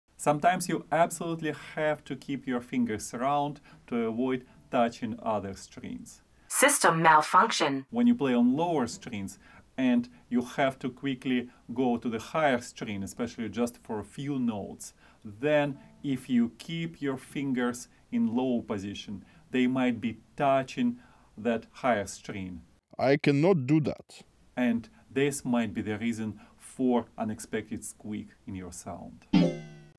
Why do I Squeak SO MUCH?
In this insightful YouTube short, we delve into a crucial aspect of cello playing – maintaining the correct finger shape to avoid unwanted string contact. Discover the importance of keeping your fingers rounded, especially when transitioning from lower to higher strings for brief notes. This technique prevents your fingers from inadvertently touching other strings, a common issue that can lead to unexpected squeaks in your sound. Ideal for both beginners and advanced players, this video offers practical advice to refine your playing technique and ensure a clean, clear sound on your cello. Tune in for a concise and effective lesson to enhance your cello performance! 🎻🎶👆